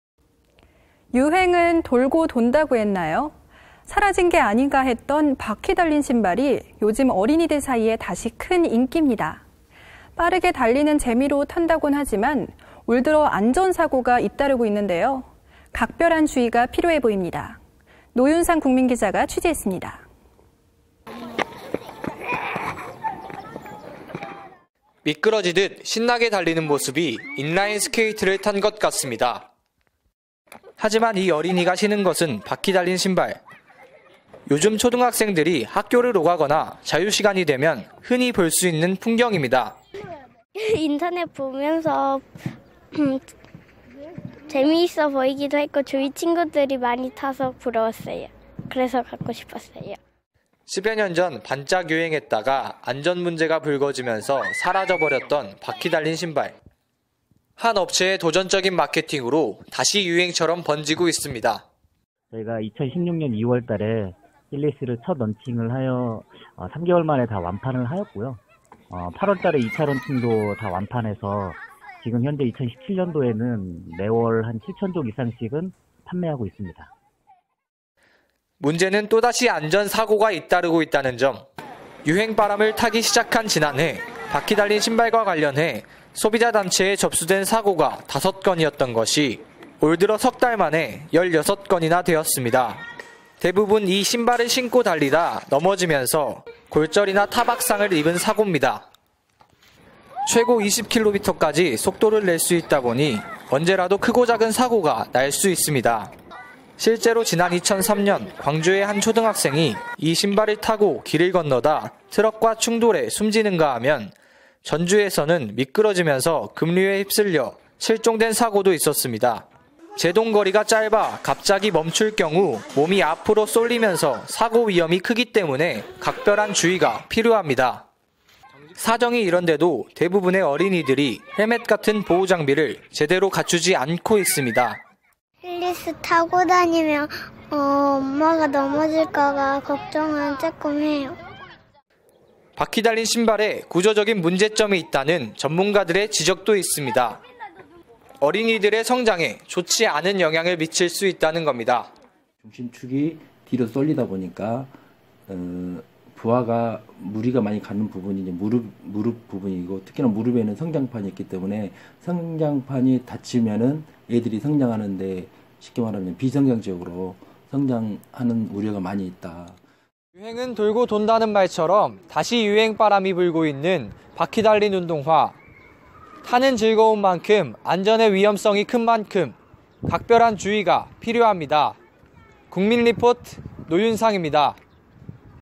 바퀴 달린 신발 또 유행…안전사고 우려
앵커:
유행은 돌고 돈다고 했나요.
사라진게 아닌가 했던 바퀴 달린 신발이 요즘 어린이들 사이에 다시 큰 인기입니다.
빠르게 달리는 재미로 탄다고 하지만 올들어 안전사고가 잇따르고 있는데요, 각별한 주의가 필요해 보입니다.
노윤상 국민기자가 취재했습니다.
 기사내용:
신나게 달리는 모습이 인라인스케이트를 탄 것 같습니다.
하지만 이 어린이가 신은 것은 바퀴 달린 신발, 요즘 초등학생들이 학교를 오갈 때나 자유시간이 되면 흔히 볼 수 있는 풍경입니다.
인터뷰: 이서원 / 광주광역시 남구
“인터넷 보면서 -바퀴 달린 운동화가- 재미있어 보이기도 했고 주변 친구들도 많이 타서 부러워서 신발을 샀습니다.”
10여 년 전 '반짝 유행'했다가 안전문제가 불거지면서 사라져버렸던 바퀴 달린 신발, 한 업체의 도전적인 마케팅으로 다시 유행처럼 번지고 있습니다.
전화인터뷰: 원용재 이사 / 바퀴 달린 신발 판매업체
“지난해 2월에 처음 입고해 다 판매했고 8월에도 추가 입고해 모두 판매됐습니다. 현재 매장에서 한 달에 7천 켤레 이상 판매되고 있습니다.”
문제는 또다시 안전사고가 잇따르고 있다는 점, 유행 바람을 타기 시작한 지난해 바퀴 달린 신발과 관련해 소비자단체에 접수된 사고가 다섯 건이던 것이 올들어 석 달 만에 열여섯 건이나 됐습니다.
대부분 이 신발을 신고 달리다 넘어지면서 골절이나 타박상을 입은 사고입니다.
최고 20km까지 속도를 낼 수 있다 보니 언제라도 크고 작은 사고가 날 수 있습니다.
실제로 지난 2003년 광주의 한 초등학생이 이 신발을 타고 길을 건너다 트럭과 충돌해 숨지는가 하면 전주에서는 미끄러지면서 급류에 휩쓸려 실종된 사고도 있었습니다.
제동거리가 짧아 갑자기 멈출 경우 몸이 앞으로 쏠리면서 사고 위험이 크기 때문에 각별한 주의가 필요합니다.
사정이 이런데도 대부분의 어린이들이 헬멧 같은 보호장비를 제대로 갖추지 않고 있습니다.
인터뷰: 이민서 / 광주광역시 남구
“바퀴 달린 운동화를 타고 다니면 엄마가 넘어질까 봐 걱정합니다.”
바퀴달린 신발에 구조적인 문제점이 있다는 전문가들 지적도 있습니다.
어린이들의 올바른 성장에 좋지 않은 영향을 미칠 수 있다는 겁니다.
인터뷰: 나승희 교수 / 조선이공대 스포츠재활과
“중심축이 뒤로 쏠리다 보니 무리가 많이 가는 부분이 무릎 부분인데 특히나 무릎에는 성장판이 있기 때문에 성장판이 닫히면 비정상적으로 성장할 우려가 있습니다.”
현장멘트:
유행은 돌고 돈다라는 말처럼 다시 유행 바람이 불고 있는 바퀴달린 운동화.
타는 즐거움 만큼 안전 위험성이 큰 만큼 각별한 주의가 필요합니다.
국민리포트 노윤상입니다.